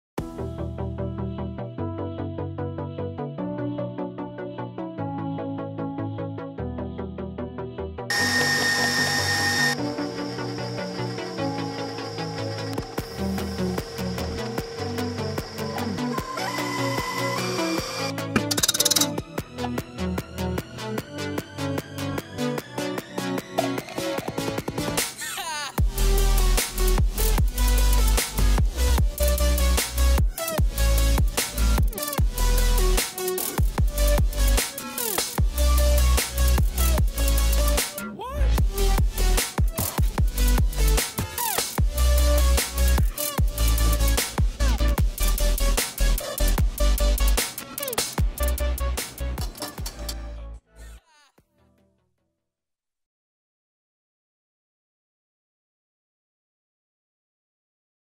How To : Blueberry Sponge
Chef Eric Bertoia shares a quick demo of interesting spin on a pastry staple.  For the recipe, ingredients and all of the details,